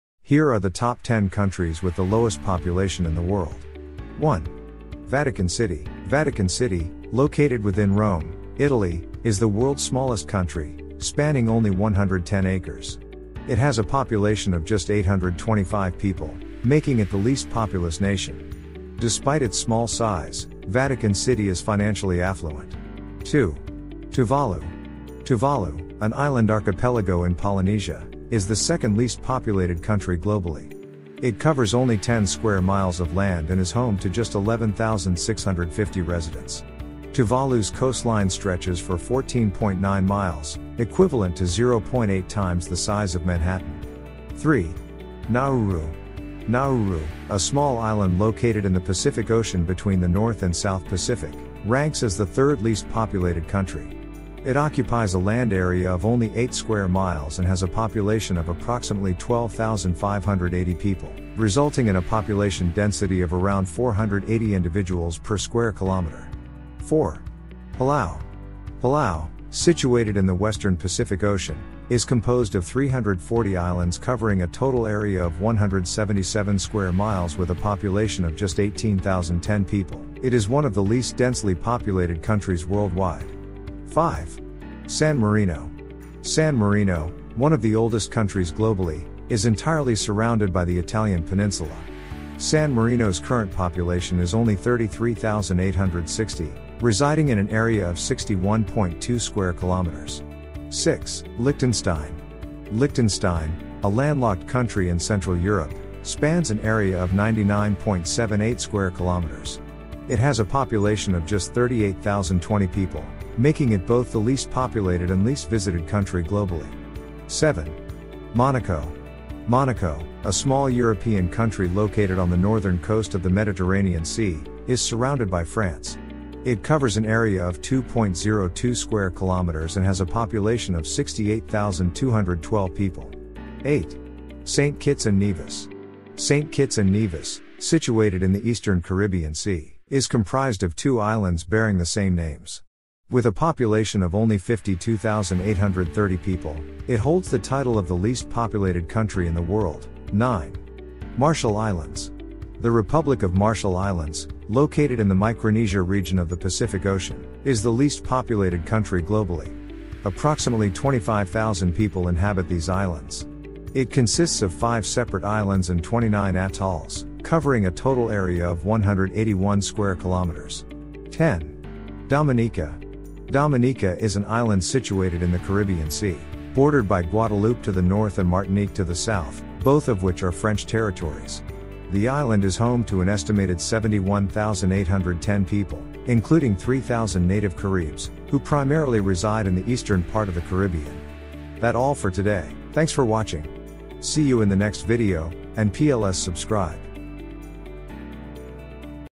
"Discover the World's Hidden Gems: Top 10 Countries with the Lowest Population
Discover the hidden gems of the world in this captivating video on the top 10 countries with the lowest population. Join us as we take a journey through these lesser-known destinations, showcasing their unique cultures and natural wonders. From breathtaking landscapes to rich traditions, you'll be amazed by what these overlooked countries have to offer. Whether you're a travel enthusiast or just curious, this video is sure to inspire your wanderlust. Don't miss out on this opportunity to broaden your horizons and explore the road less traveled!